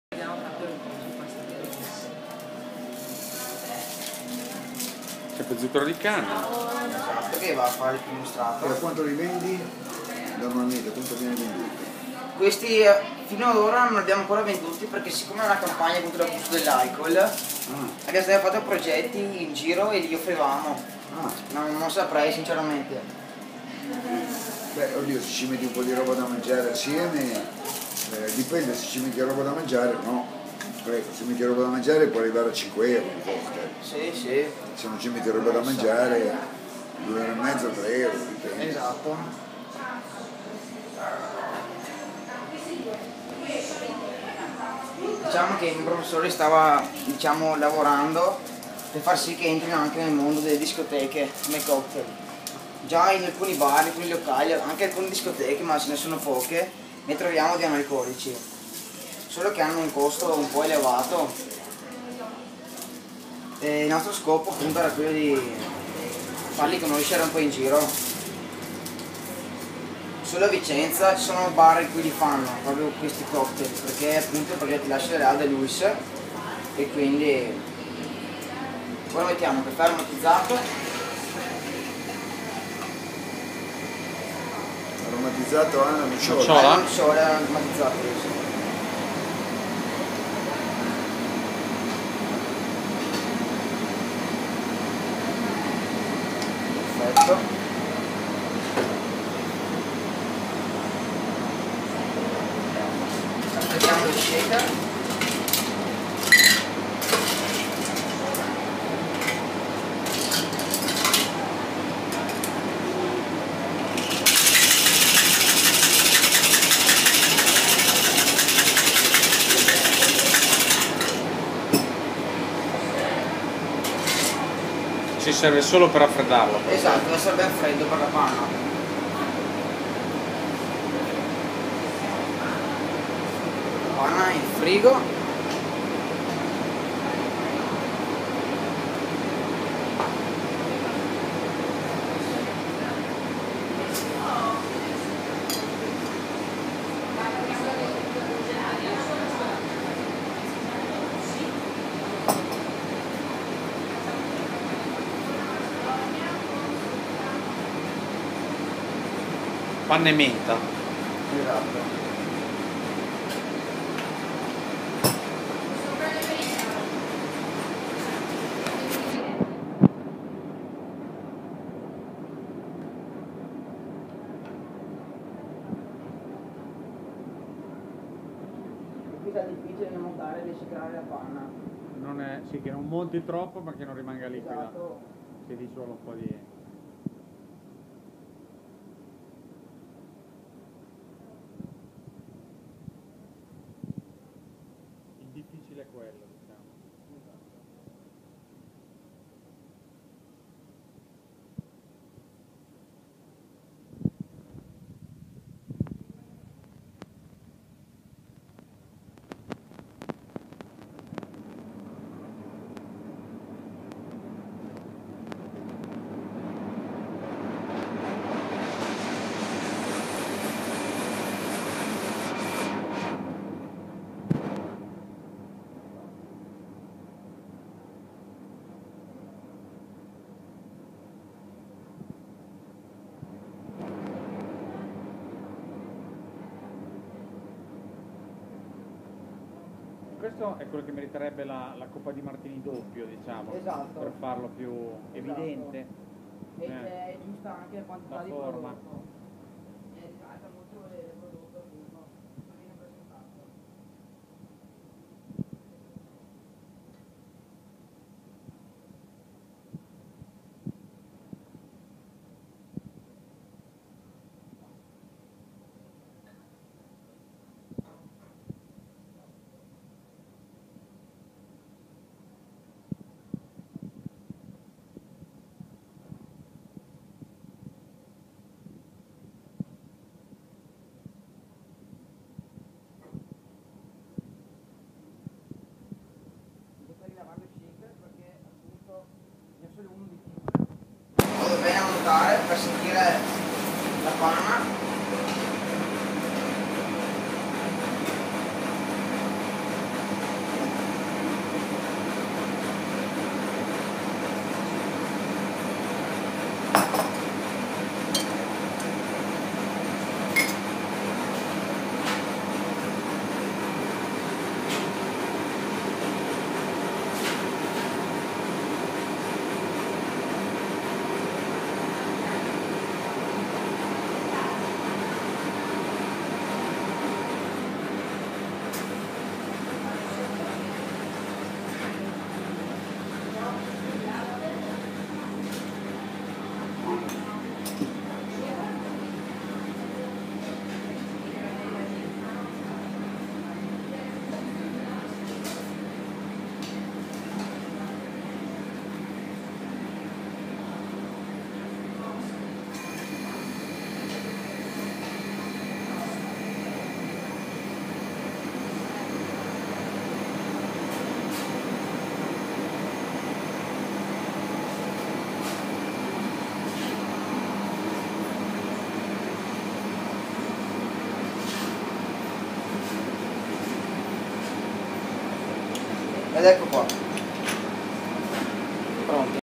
L'APERYOGURT anche in yogurteria i cocktail
Un idea per l'aperitivo e non solo. I COCKTAIL analcolici...
Libera la tua fantasia!!